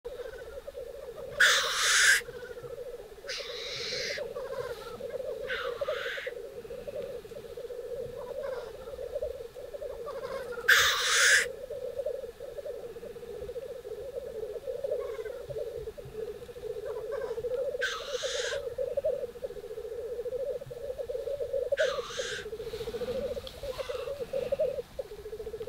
Black Grouse lek 4k
A short video from a Black Grouse lek up in northern Sweden. The video is recorded in April 2021 using a DSLR and a shotgun mic.

I really like the bubbling sound that the male produce as that often is the first sound you hear when you are waiting for them to arrive on the lek site.